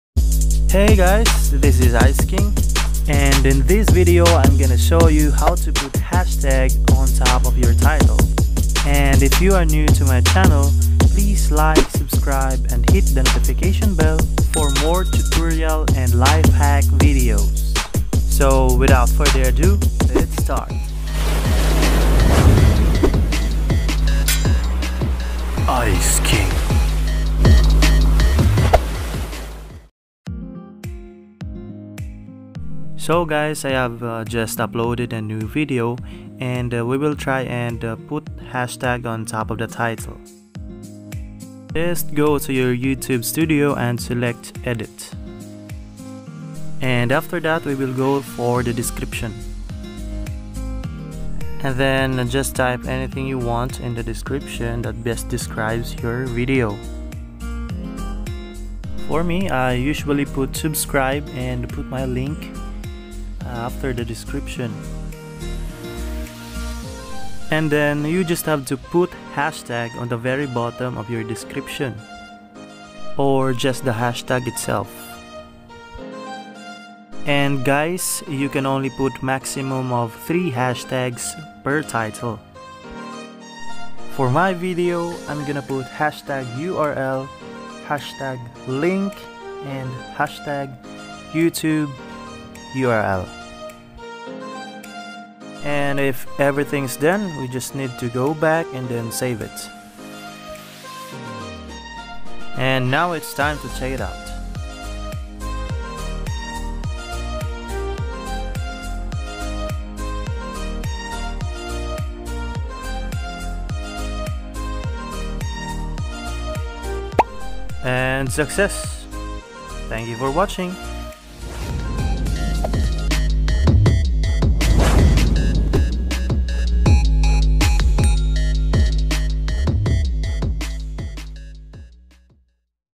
How to Add Hashtags Above Youtube Video Title
Tutorial on how to put Hashtags above YouTube Videos using mobile phones.

YouTube Hashtags, often referred to with the “#” symbol, are tags added to video titles and descriptions that denote a video's topic or category. According to YouTube, hashtags can improve a video's discoverability on the YouTube platform.